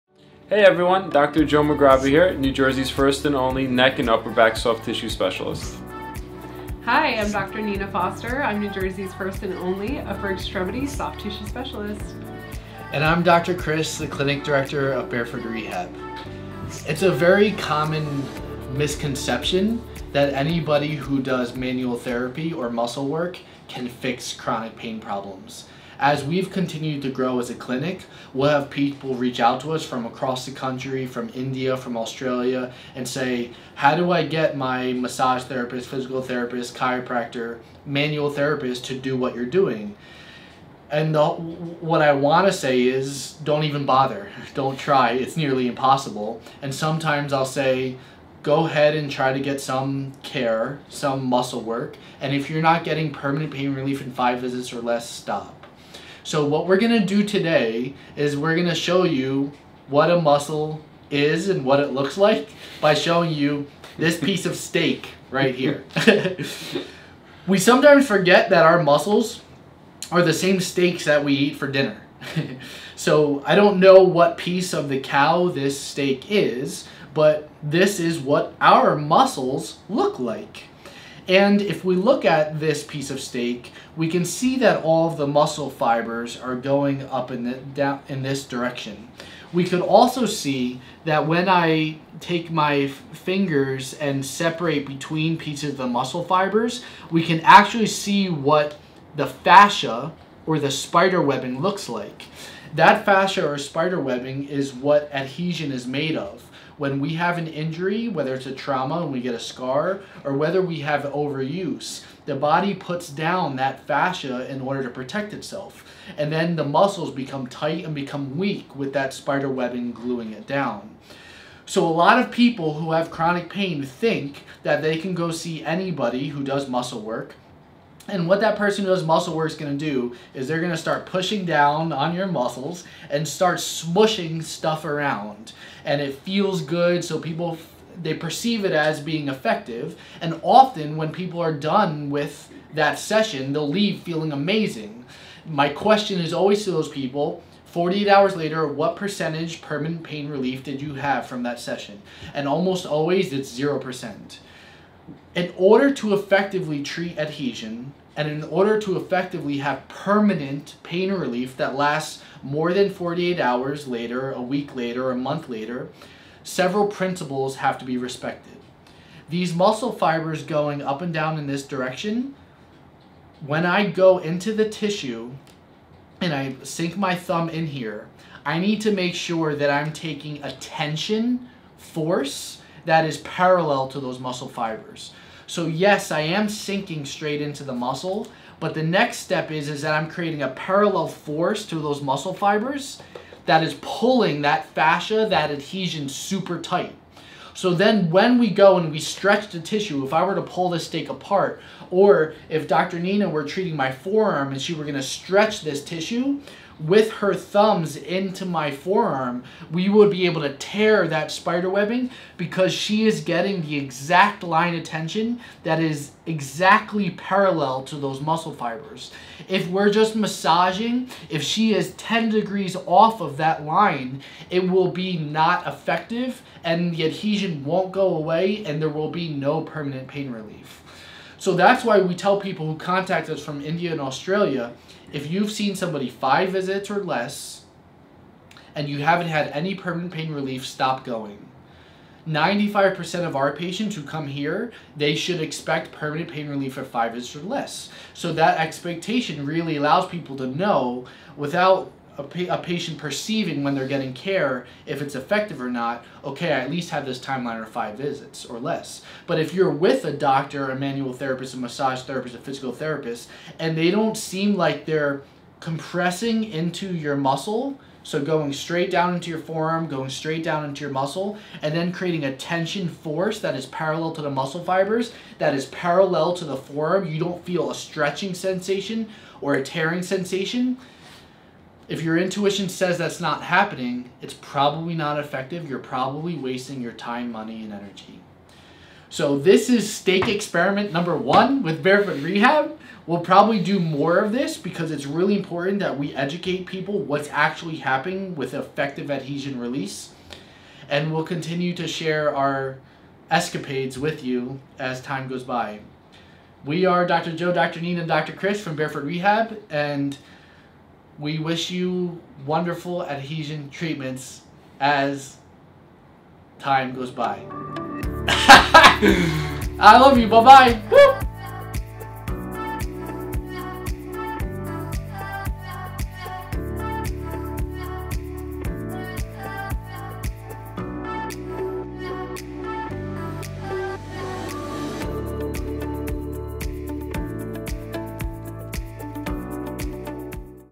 The Steak Experiment #1: Manual Therapy for Chronic Pain Relief
The Steak Experiment #1: Why doesn't all Manual Therapy work for Chronic Pain Relief?

People want to know, "Why can't I see any massage therapist, physical therapist, or chiropractor to get PERMANENT pain relief?"

In this video, Dr. Joe, Dr. Nina, and Dr. Chris discuss why smushing muscles (most manual therapy you'll get at any therapy) doesn't result in PERMANENT pain relief after 48 hours have passed by.

Our technique at Barefoot Rehab, Manual Adhesion Release, is what allows us to play by the 5 Visit Rule that allows you to know, with 95% certainty, that you can expect some % of PERMANENT relief by then.

What makes our technique work is physics. Our manual therapy is a tension force, not a compression force that works. Adhesion needs to be pulled apart. Once you're glued down enough by adhesion, no amount of stretching or just smushing or massaging will fix it. Evidence of this fact is your more than 6 months of pain!

▬ More on "Manual Therapy" ▬▬▬▬▬▬▬▬▬▬▬▬

▬ Social Media ▬▬▬▬▬▬▬▬▬▬▬▬▬▬▬